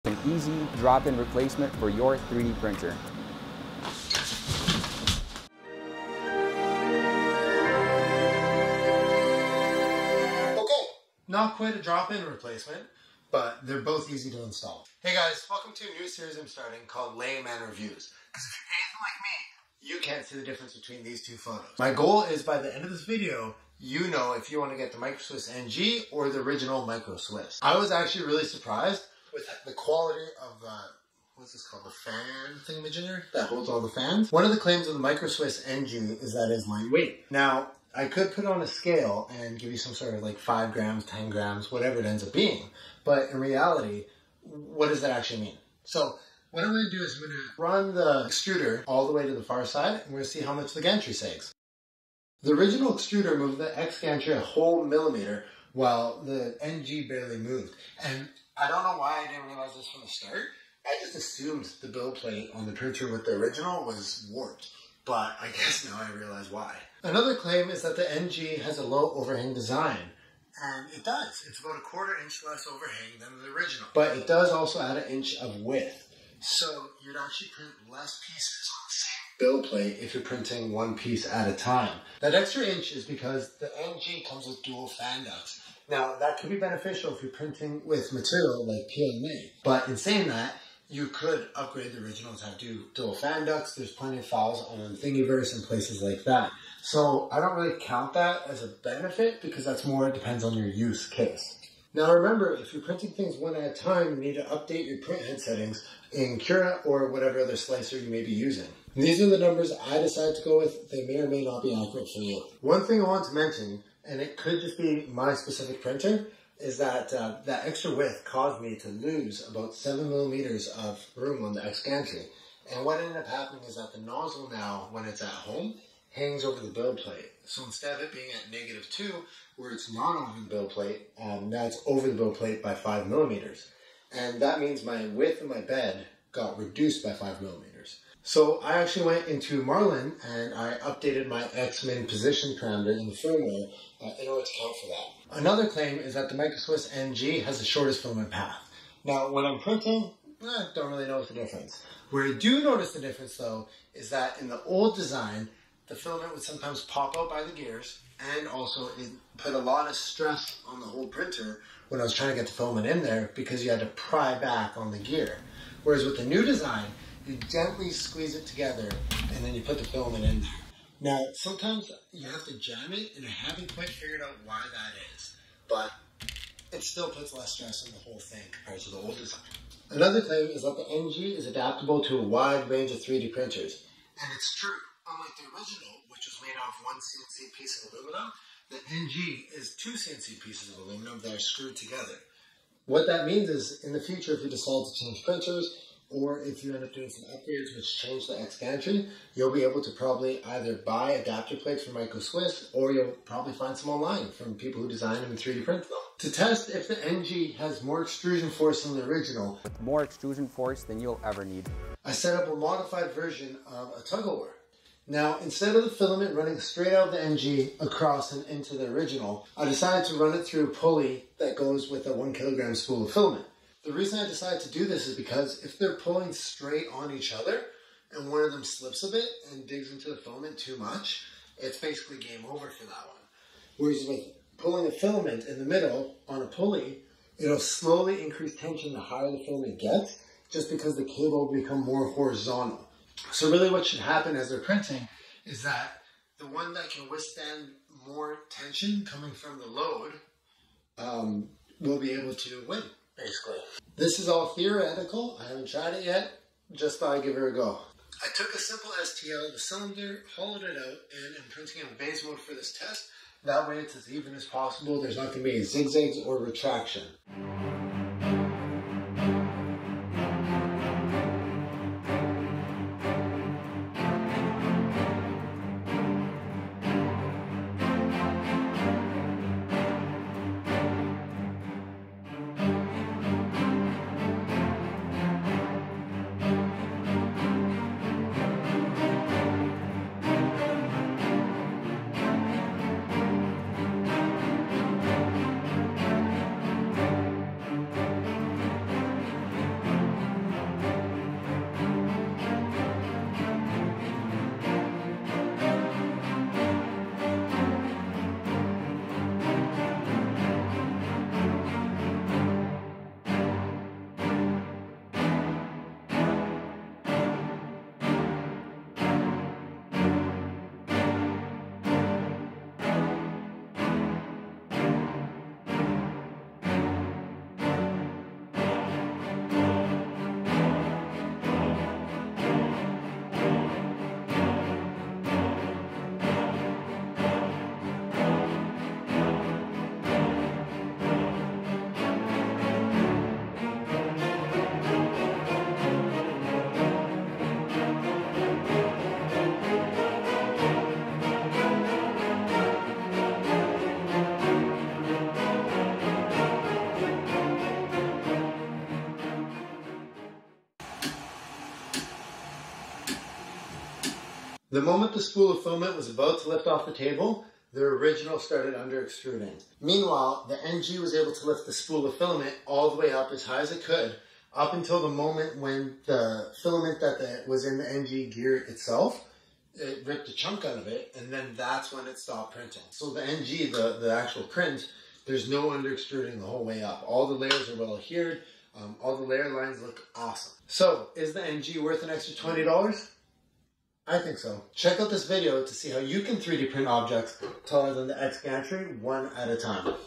Is the Microswiss NG worth the Money? | Layman Reviews
Is the Microswiss NG worth buying over the Original Microswiss direct drive extruder with all metal hot end? In this video I try to show the pros and cons of the Microswiss NG in layman terms.

I also am using an Ender 3 with an Ender Extender 400XL Kit. I've had the kit for over a year and love it!

00:00 Drop in Replacement
00:14 Layman Reviews
00:27 Initial Impressions
00:34 Does the weight matter?
01:06 Low Overhang Dimensions and Details
02:25 Shortest Filament Path
02:59 Is the NG modular?
03:26 Tug Of War Explanation
04:27 Tug Of War!
06:30 Is the NG Worth it?